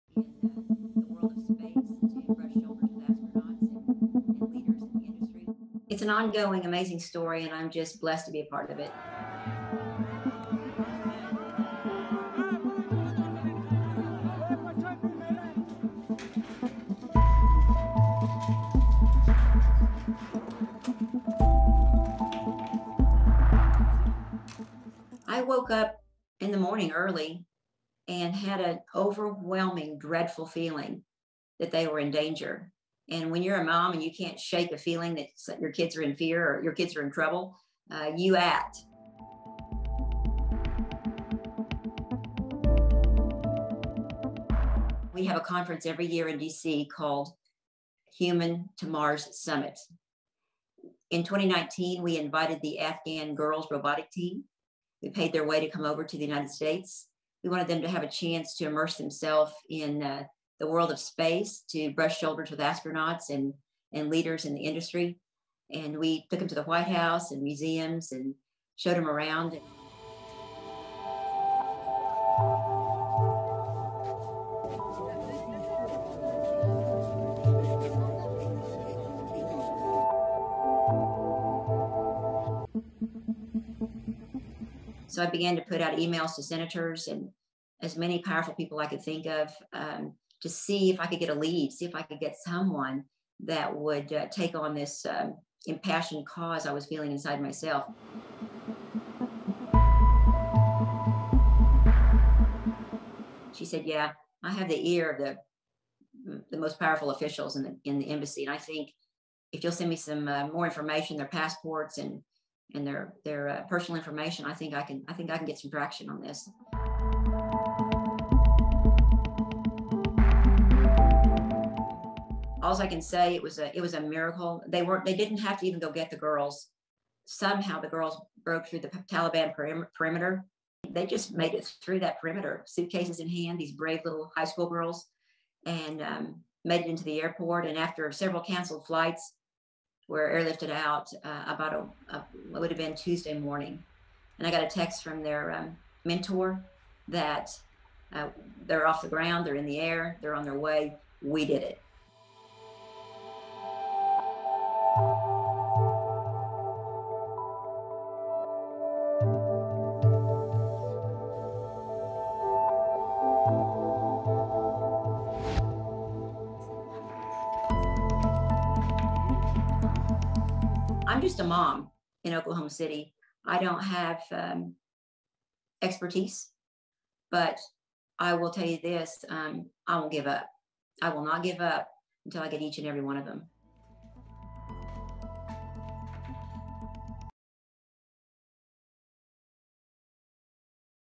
‘Samo mama’ pomogla bjeg stručnjakinja za robotiku iz Avganistana
Allyson Reneau, majka 11 djece i stručnjakinja za američku svemirsku politiku, poželjela je dobrodošlicu timu afganistanskih djevojaka za robotiku kada su putovale u Sjedinjene Američke Države 2019. na tehnološku konferenciju.